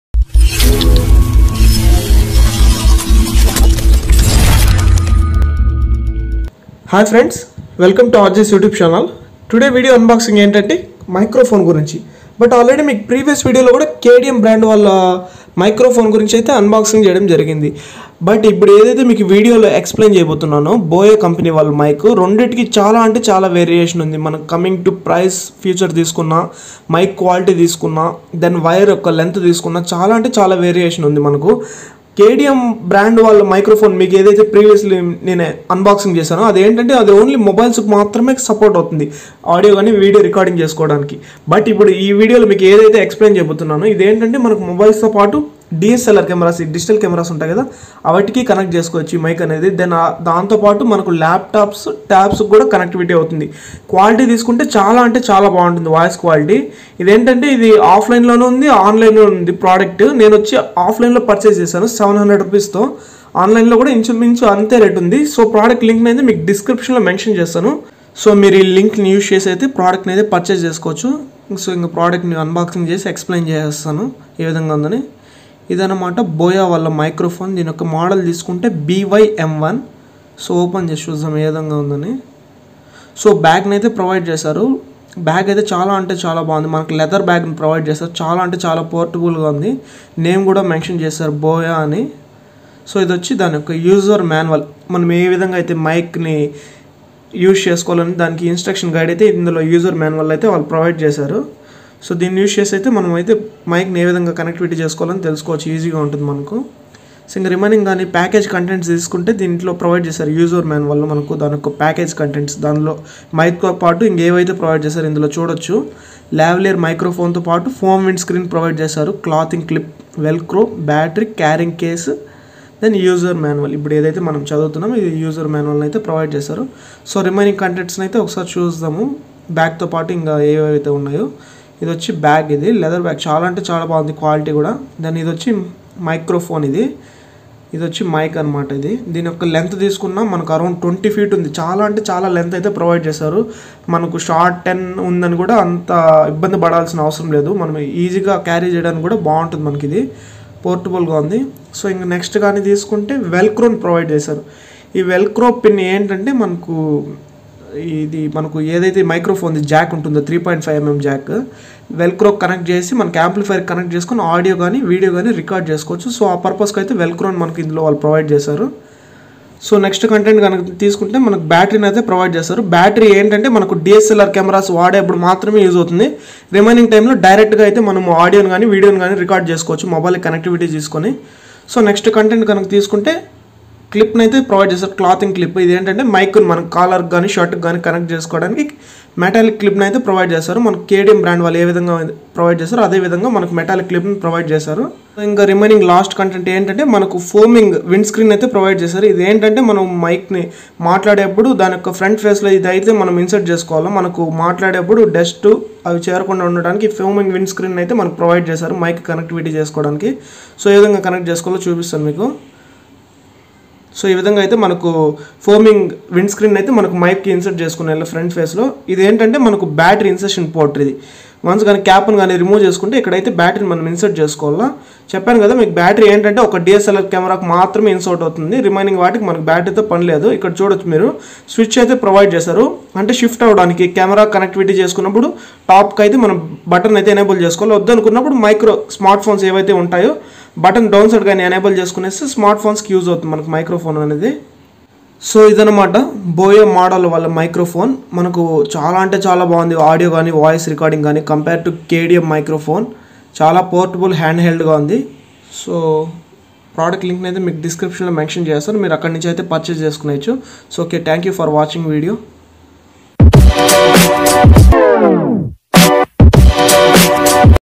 Best Mic For YouTube Videos | Best Budget Mic | Boya BY-M1 | Unboxing & Review in Telugu
Best Microphone For Every Youtubers || BOYA BY M1 Review in TELUGU

Boya By M1 Unboxing . This is the best lapel mic for youtube. It is Omnidirectional Lavalier Condenser Microphone.

Best Mic for YouTubers, for Video voiceovers, for short films. This Mic is a very budget-friendly and cheapest Mic that provides studio-level audio if you can manage to record without noise.

Tip: Battery కేవలం switch on చేయటానికి మాత్రమే ఉపయోగ పడుద్ది, smartphones లో వాడాలంటే battery అవసరం లేదు.
The battery is only needed when you are switching to the Camera's side, so that means the battery is not required to use with smartphones.